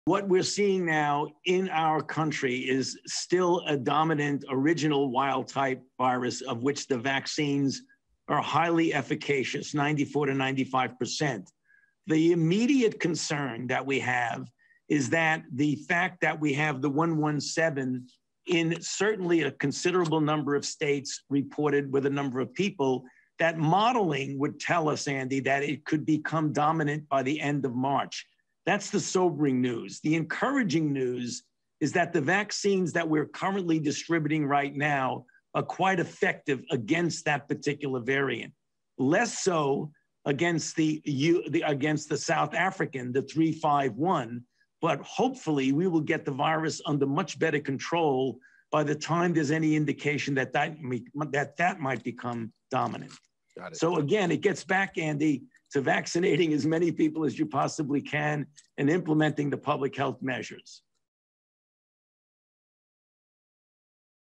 VIDEO NOW: Dr. Fauci discusses COVID variants and vaccine effectiveness
Dr. Anthony Fauci discusses COVID-19 variant concerns and vaccine effectiveness during a White House COVID-19 response team briefing on Monday, Feb. 8